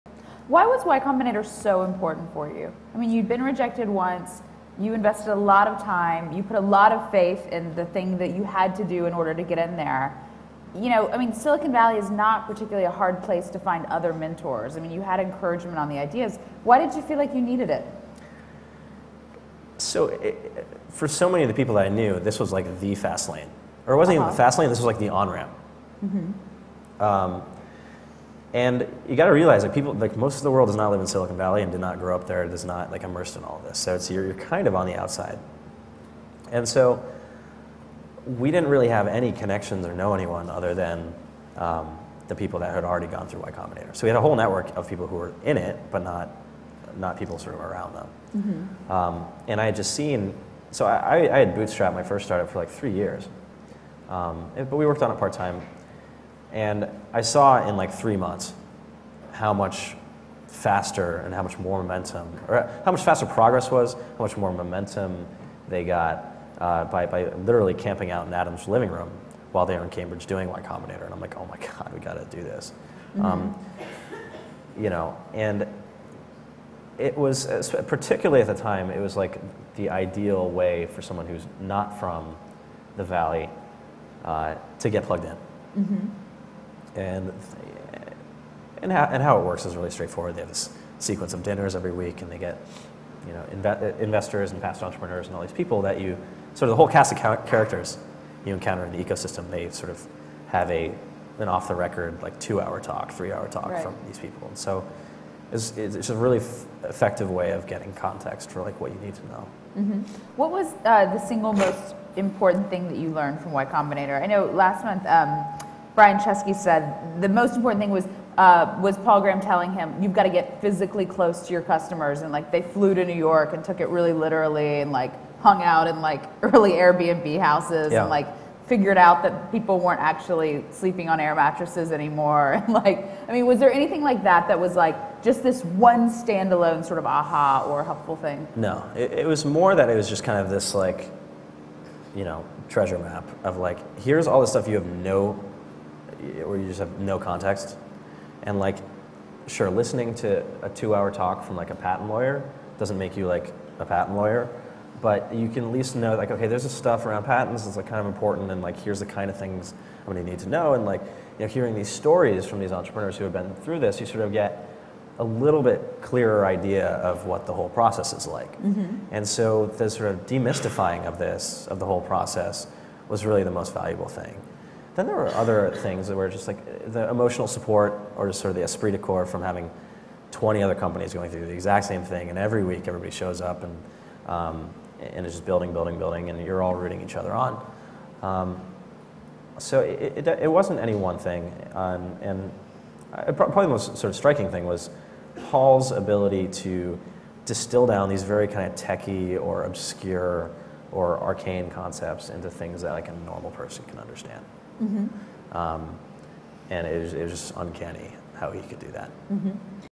PandoMonthly: Why Y Combinator was so important for Dropbox
Dropbox CEO Drew Houston talks about what he learned from Paul Graham and from being in Y Combinator.